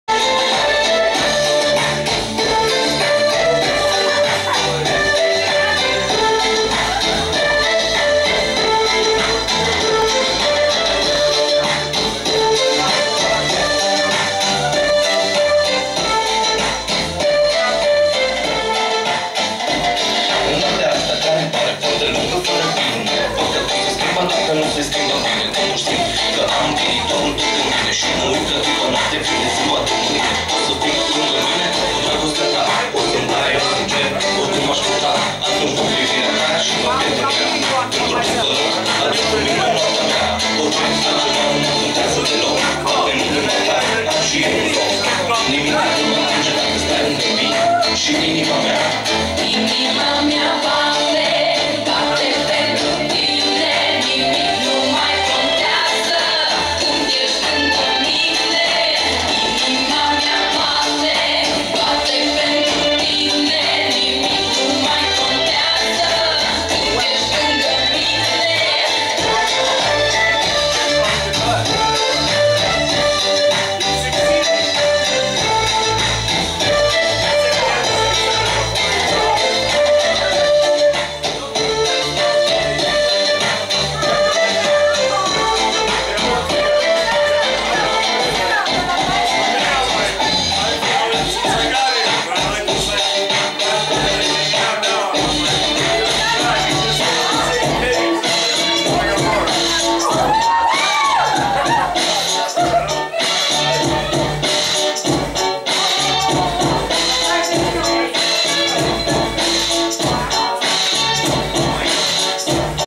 Retro buli a Nomad Gastro Pubban
Első alkalommal szerveztek retro bulit a kolozsvári Nomad Gastro Pubban. Folyatás következik.